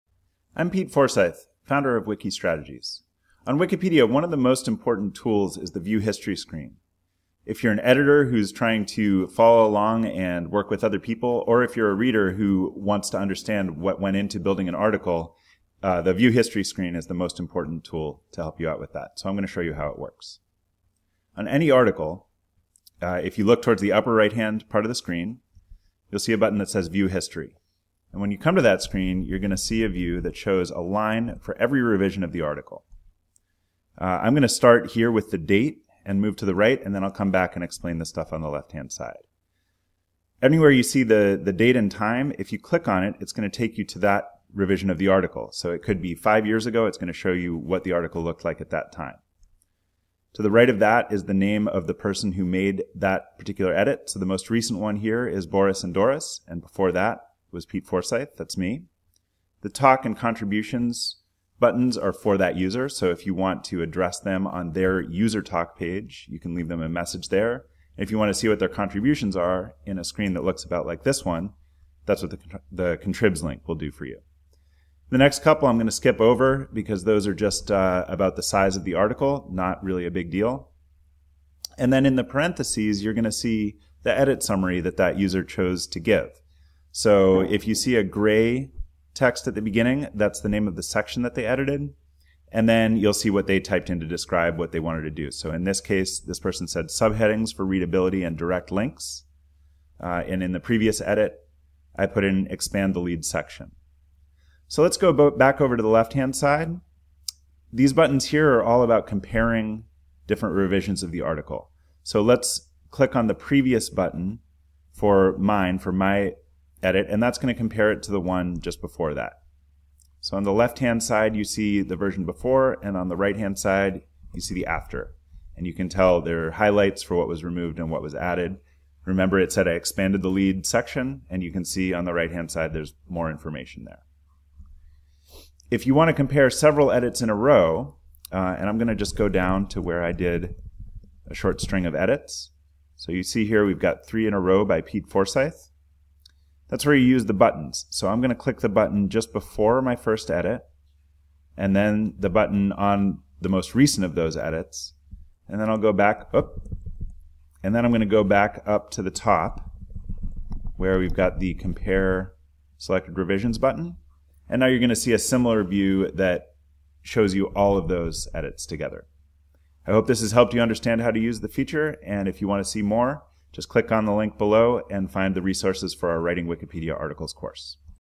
Using Wikipedia's View History screen
The "view history" screen is a vital part of Wikipedia's software, and can help both editors and curious readers of Wikipedia better understand how an article evolved to its current state.

This video is part of the free online course Writing Wikipedia Articles; visit the course page for more learning resources about Wikipedia.